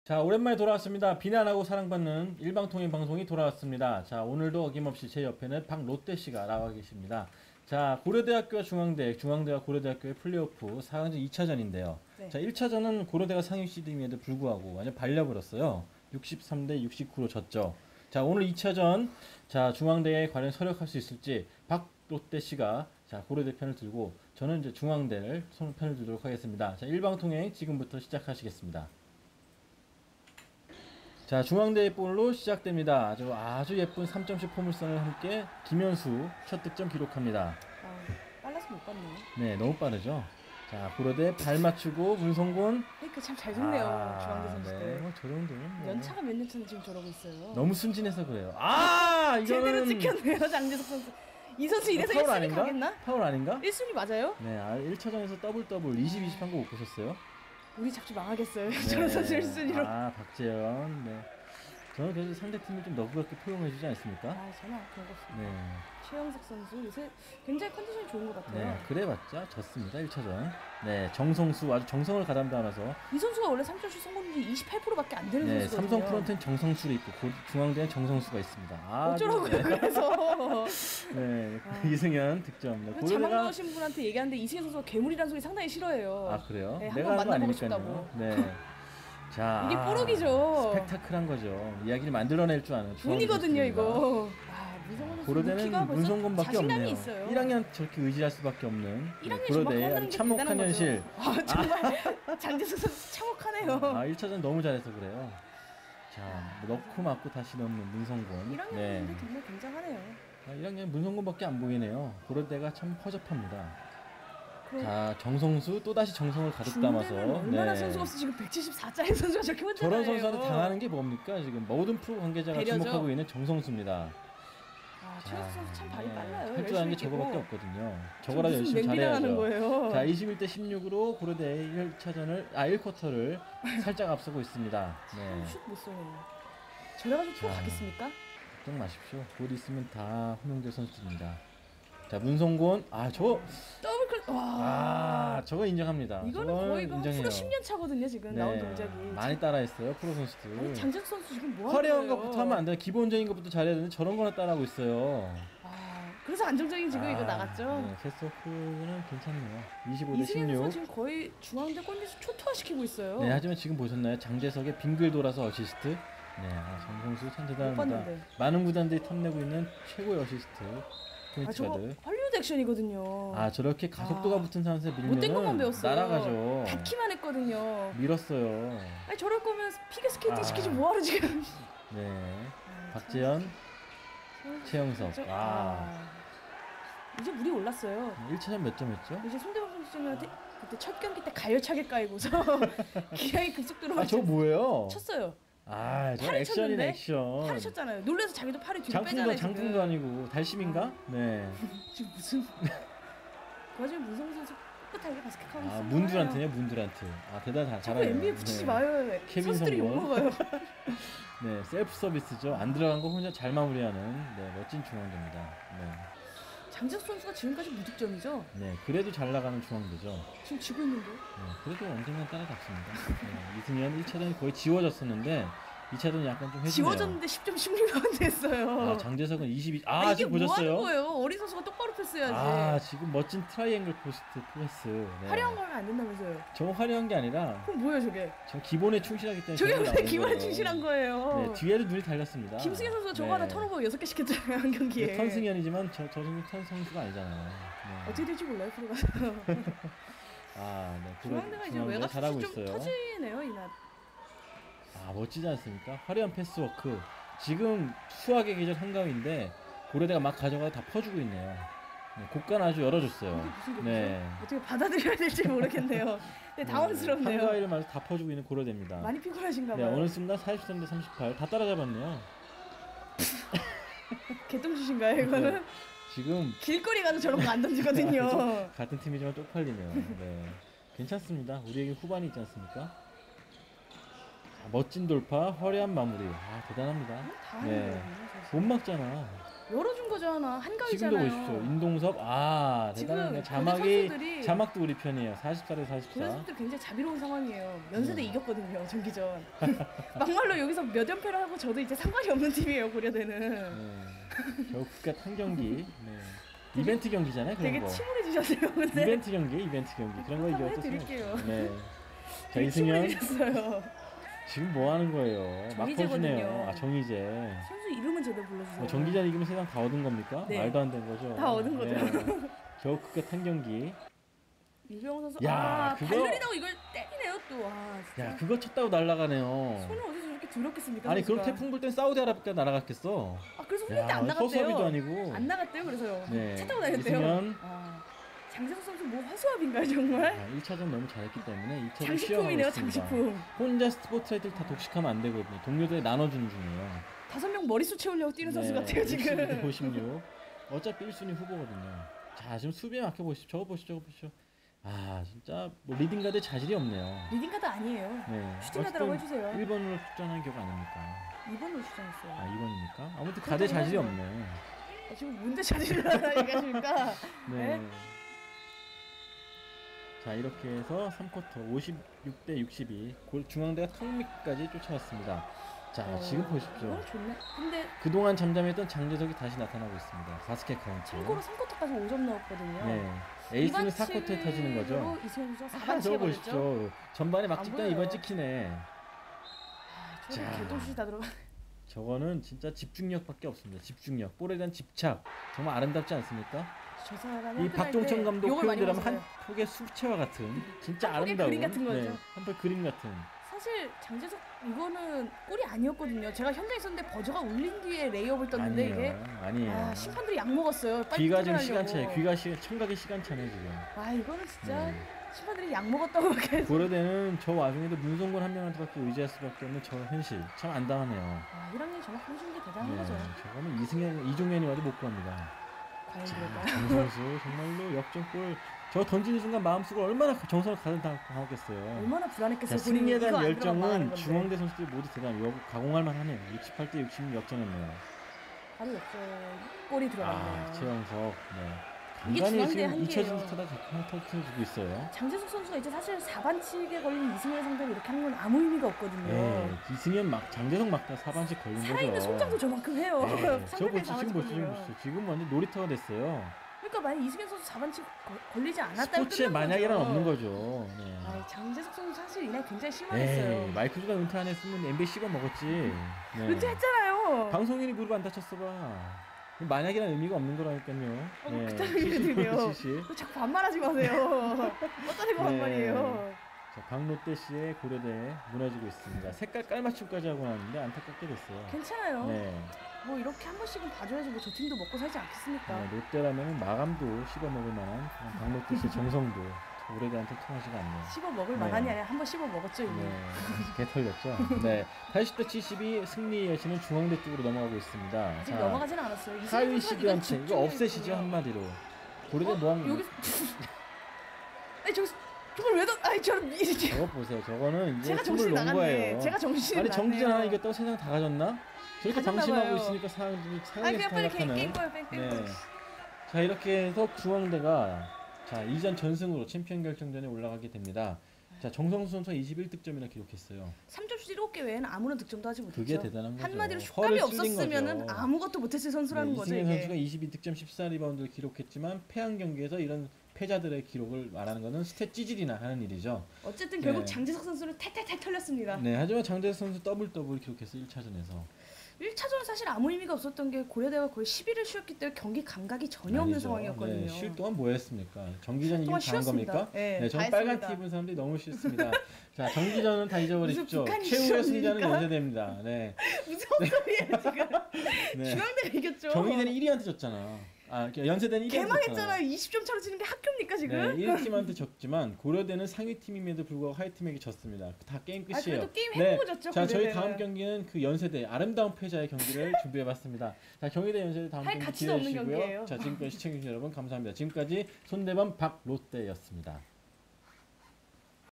대학리그4강 고대 중앙대 일방통행 최종본
그야먈로 '마지막 승부'를 떠올리게 합니다. 스포츠에서 진정한 승부는 5판 3승에서 나오는 법입니다. 올시즌 라이벌 관계를 형성한 두팀이 벌이는 그동안의 치열한 승부를 떠올리게 합니다.

저희는 25일 중앙대 안성캠퍼스 체육관에서 벌어진 2012 KB국민은행 대학농구리그 4강 플레이오프 2차전 중앙대와 고려대의 경기를 준비했습니다. 올해 이 두팀은 그야말로 라이벌 관계를 형성하면서 치열한 승부를 벌여왔습니다. 특히, 어느 한팀도 10점차 이상의 승부를 허용하지 않을 만큼 치열함의 극치를 보여줬습니다.

이런 상황에서 24일 고려대 홈인 화정체육관에서 벌어진 4강 플레이오프 1차전 승부, 원정팀임에도 불구하고 중앙대가 먼저 기선을 잡았습니다. 장재석이 이승현을 압도한 경기였다고 할 수 있습니다. 2차전에서는 절치부심한 이승현이 장재석을 상대로 어떤 모습을 보여줄까요? 아울러 김현수를 필두로 한 중앙대의 가공할만한 외곽포를 고려대에서는 어떻게 대비할 것인지도 관심이 갑니다. 아울러 고려대가 대학리그때의 모습을 그대로 보여줄 수 있다면 바랄 것이 없습니다.

올시즌 마지막 승부라고 할 수 있는 중앙대와 고려대의 승부, 신은 어느 팀에게 손을 들어드릴까요?

출연 : 손대범 편집장, 박단비 기자
영상촬영/ 편집 : 김남승, 권민현, 박선희 영상/중계팀 기자